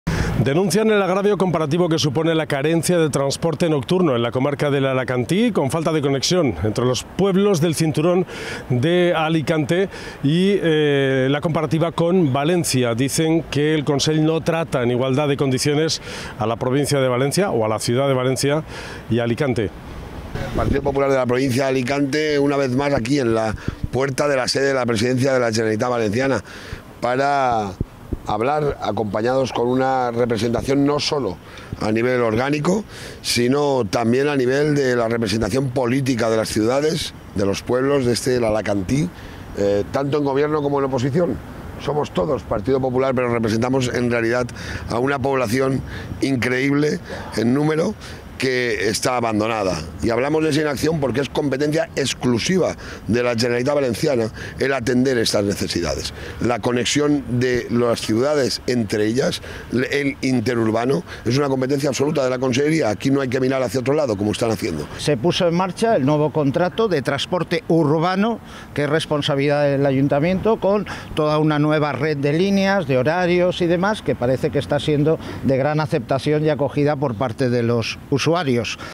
Toni Pérez y José Antonio Rovira anuncian medidas por la huelga de autobuses en l'Alacantí
Toni Pérez y José Antonio Rovira anuncian medidas por la huelga de autobuses en l'Alacantí junto a alcaldes y portavoces del PP en la comarca.